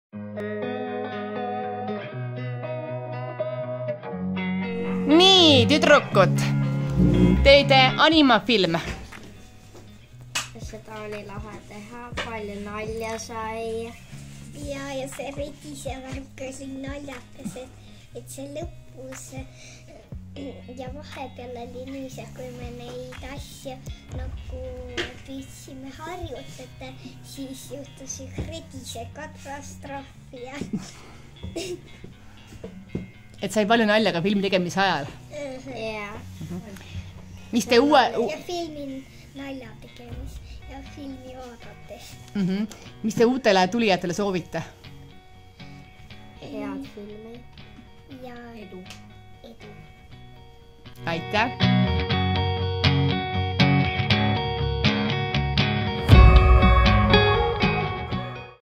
Miks osaleda Pärnu õpilaste filmifestivalil? - Pärnu Vabakool animafilm "Friida"
Selles videos räägivad väikesed filmitegijad, mida nemad õppisid pärnu õpilaste filmifestivalil ning miks Sina võiksid sellest festivalist osa võtta.